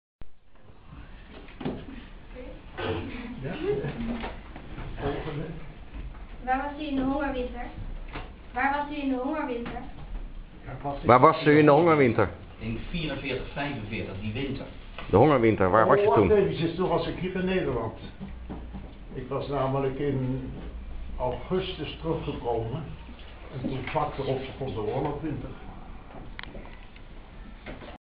Vader op Houtrustschool over zijn Duitse gevangenschap
Mijn vader vertelt op zijn gevangenschap tijdens de Duitse bezetting, omdat hij een radio in bezit had.